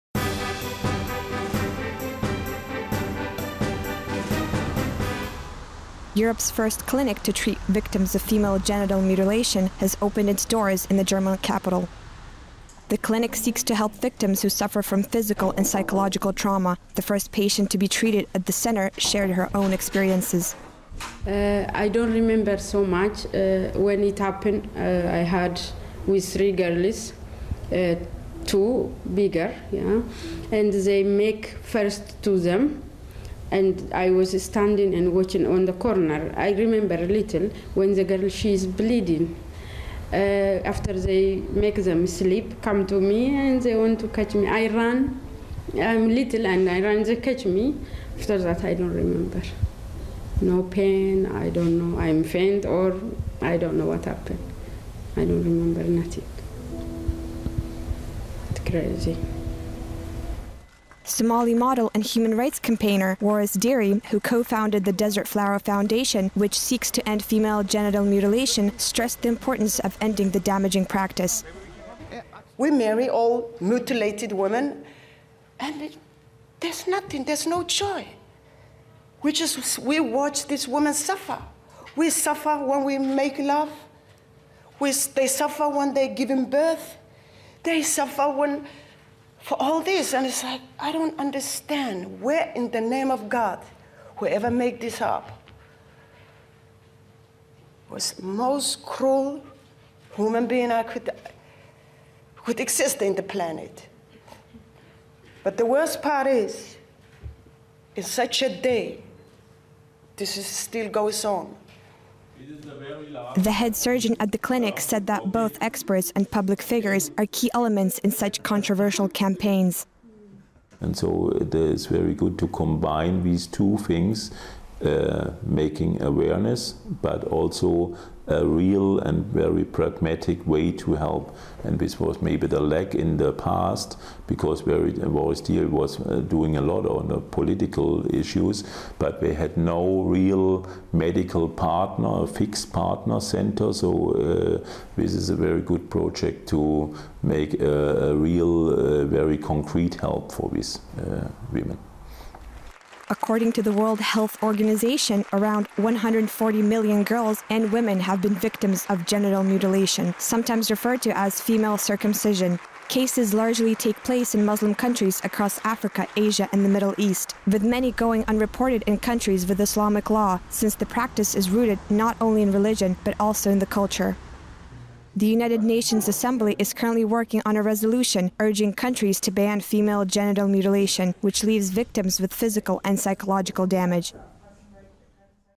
Female Genital Mutilation: Europe's first female genital mutilation clinic opens in Germany
Europe's first clinic to treat victims of female genital mutilation has opened its doors in the German capital.